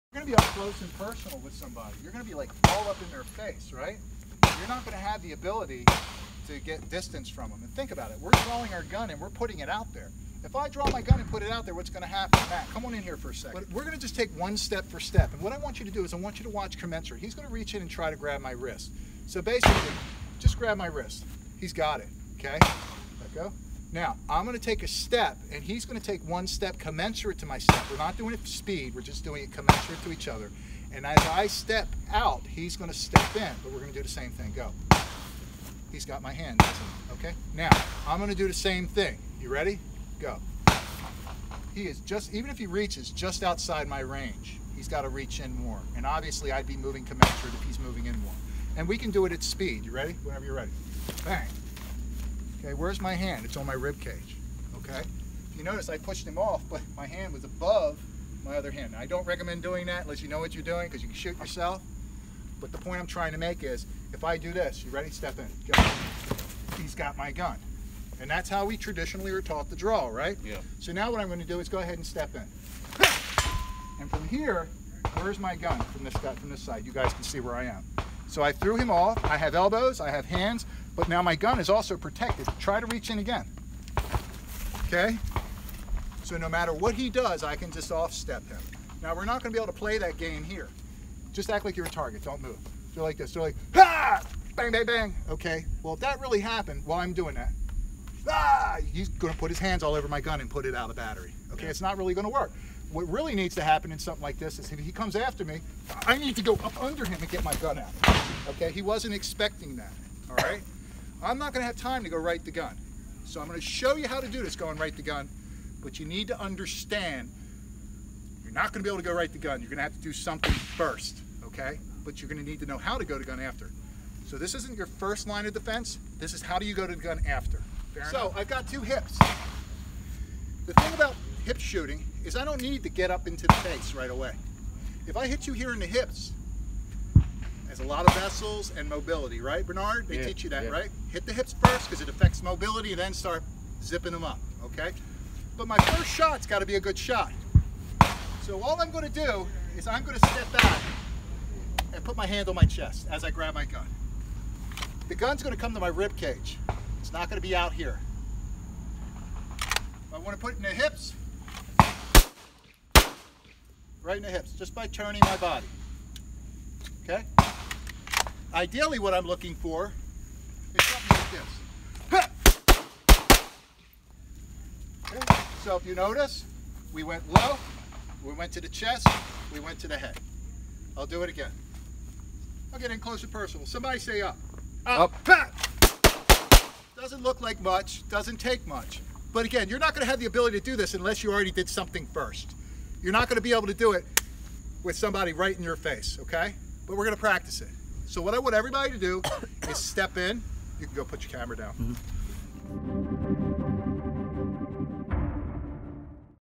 Tactical U Tibdit - Hands Before Gun - Close Range Concepts - Tactical U Firearms Trianing
Police officers and other professionals in close quarter situations many times are not going to be able to draw and shoot their firearm effectively as easily as demonstrated against a piece of cardboard in close proximity.

Most close range gunfights don’t go right to gun especially with police. Many times due to slower reaction, surprise and hesitation, the use of retention skills, take downs and striking can become extremely valuable. However, we still need to teach you how to get the gun out and work it effectively if there’s a possibility to get the gun out.

Don’t rely on these range techniques by themselves to save your life. It’s very likely your adversary could grab your weapon in close proximity, stab you, or shoot you. Remember range training is like being the best boxer in the room against the bag. Once someone starts punching back, the game is redefined.

Suggestion, take hand to hand, striking, take down and edged weapon classes from professional reputable teachers. Do not rely on your firearms instructor for prior to go to gun circumstances. I can not stress this enough. Seek out specialist in each field.

Stay humble, get training and better your chances for survival.

This is one of several techniques you’ll learn in Gunfighting 101.